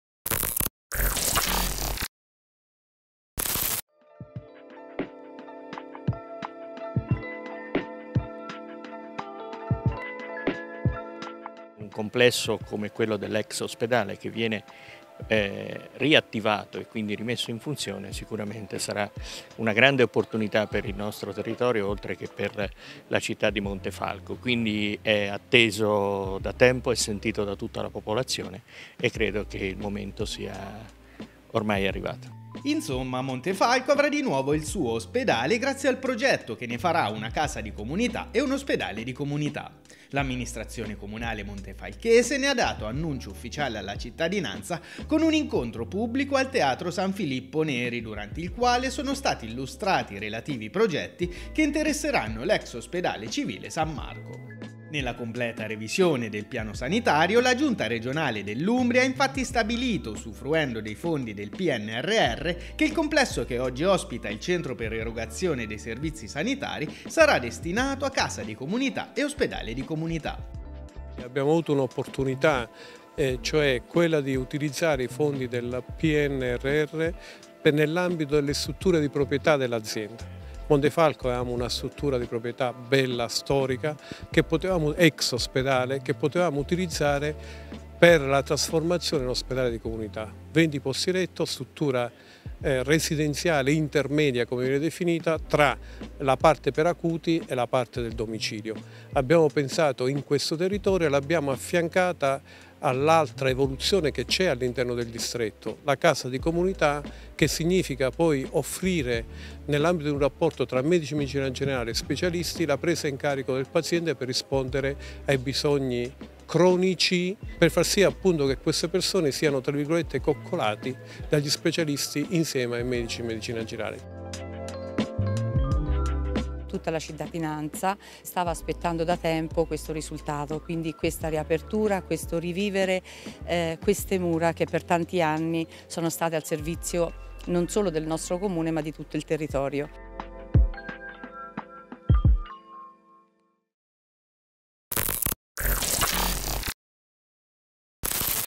Montefalco: al via il progetto per la Casa di comunità e l’Ospedale di comunità [UMBRIA NEWS]
Montefalco avrà di nuovo il suo ospedale grazie al progetto che ne farà una Casa di comunità e un Ospedale di comunità. L’amministrazione comunale montefalchese ne ha dato annuncio ufficiale alla cittadinanza con un incontro pubblico al teatro San Filippo Neri durante il quale sono stati illustrati i relativi progetti che interesseranno l’ex ospedale civile San Marco.

Nella completa revisione del piano sanitario, la giunta regionale dell’Umbria ha infatti stabilito, usufruendo dei fondi del Pnrr, che il complesso che oggi ospita il centro per erogazione dei servizi sanitari sarà destinato a Casa di comunità e Ospedale di comunità.